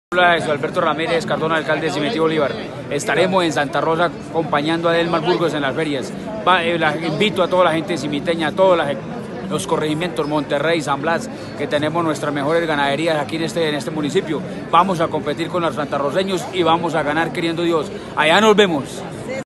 El alcalde de Simití, invita a las ferias de Santa Rosa del sur
El alcalde de Simití, Jesus Alberto Ramirez Cardona, invita a las ferias de Santa Rosa del sur